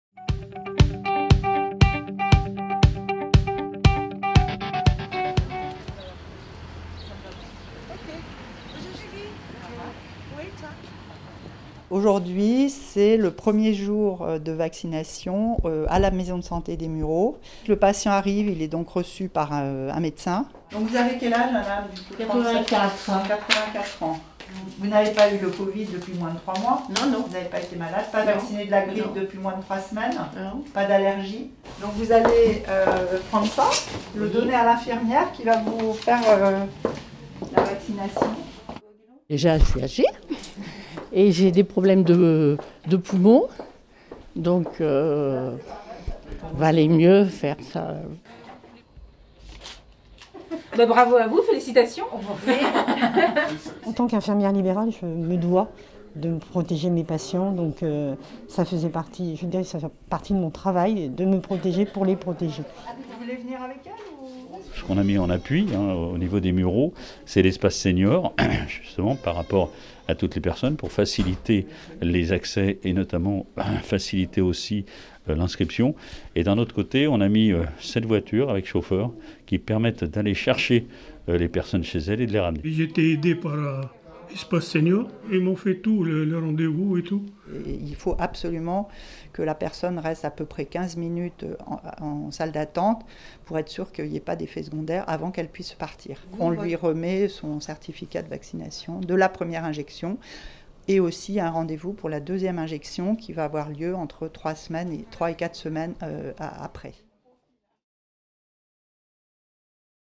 Les Mureaux - Ouverture du Centre de vaccination COVID-19 Val-de-Seine (Lundi 18 janvier 2021)
Le lundi 18 janvier 2021, le Centre de vaccination Covid-19 Val-de-Seine a ouvert ses portes au sein de la Maison de santé Philippe-Marze des Mureaux.

Consultation pré-vaccinale, première et seconde injections et surveillance post vaccinale... Tout a été pensé pour assurer aux patients une prise en charge optimale, dans un lieu de proximité.

Bien que, dans un premier temps, la vaccination s’adresse en priorité aux personnes âgées de plus de 75 ans, il est également possible pour les professionnels de santé de plus de 50 ans de se faire vacciner afin de protéger la santé de leurs patients les plus fragiles.